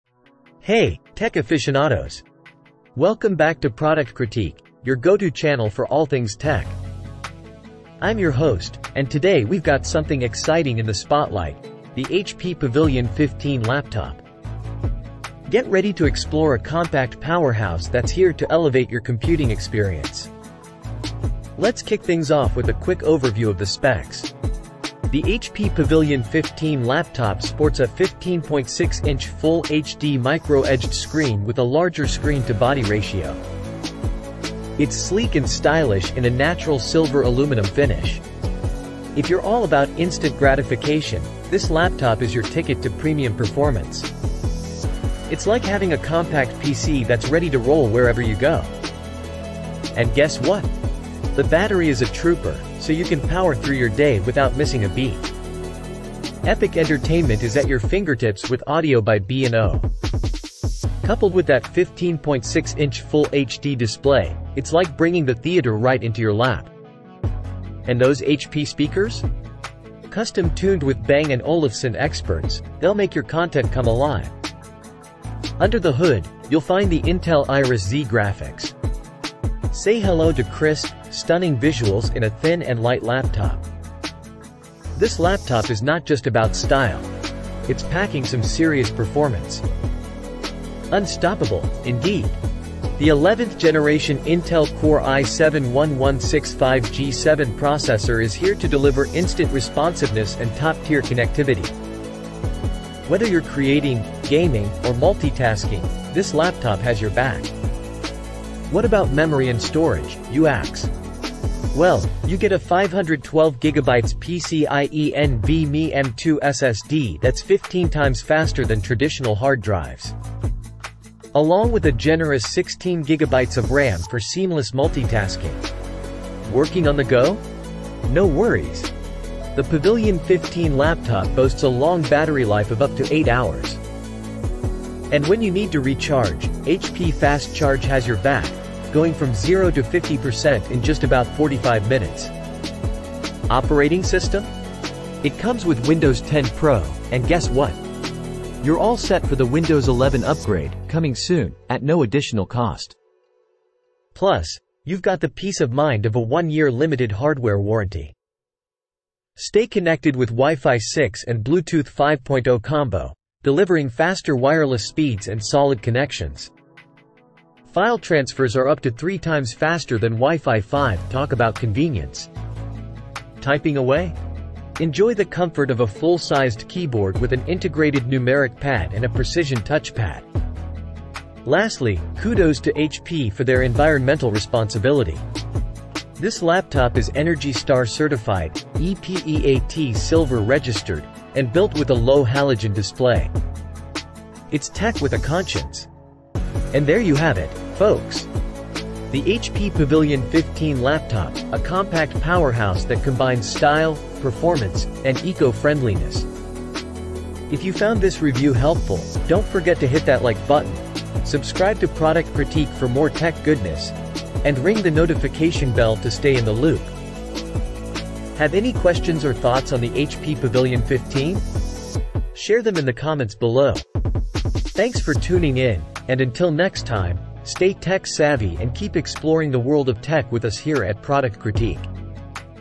HP Pavilion 15 Laptop, 11th Gen Intel Core i7-1165G7 Processor: Power Meets Portability - Review
Amazon Product Name : HP Pavilion 15 Laptop

Dive into our comprehensive review of the HP Pavilion 15 Laptop, powered by the 11th Gen Intel Core i7-1165G7 processor! In this video, we'll explore the perfect blend of performance and style this laptop offers. Discover how it handles everyday tasks, multimedia, and more with ease. We'll cover its design, features, and why it's a top choice for those seeking a versatile and powerful computing companion. If you're in the market for a laptop that delivers on all fronts, this review is a must-watch. Elevate your computing experience with the HP Pavilion 15!